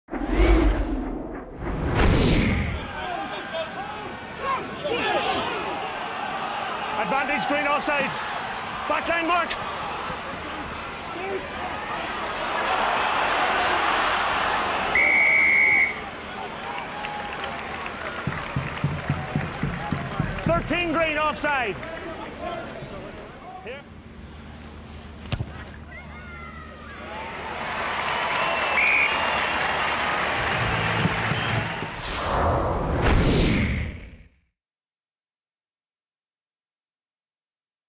Ian Madigan 2nd Penalty - Leinster v Connacht 26th Oct 2013
Leinster's Ian Madigan kicks his second penalty after a Connacht midfield offside in the Leinster v Connacht rugby match in the RaboDirect PRO12 on Saturday 26th October 2013 in the RDS, Dublin.